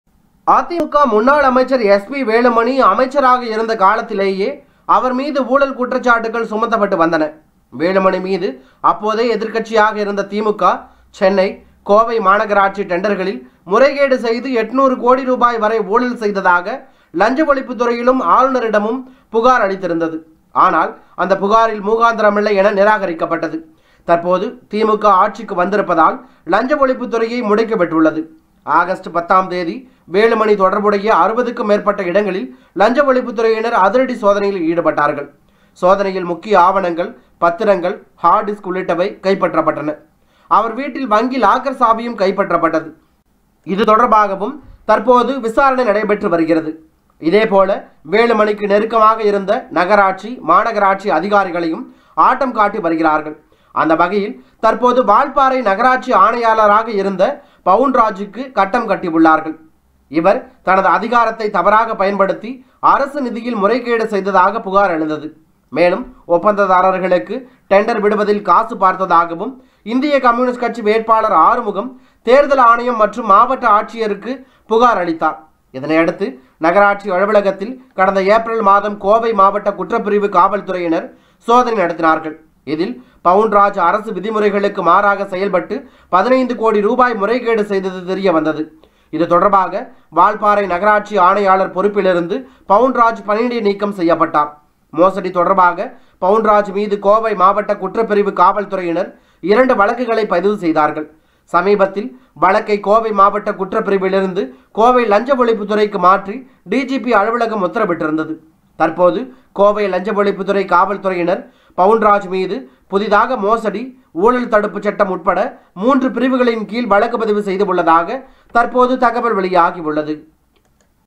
சிக்கிய முக்கிய ஆதாரம்,எஸ்.பி. வேலுமணிக்கு கட்டம் கட்டிய லஞ்ச ஒழிப்புத்துறை!, அடுத்தடுத்த பரபரப்பு
சிக்கிய முக்கிய ஆதாரம்,எஸ்.பி. வேலுமணிக்கு கட்டம் கட்டிய லஞ்ச ஒழிப்புத்துறை!, அடுத்தடுத்த பரபரப்பு | ADMK | STALIN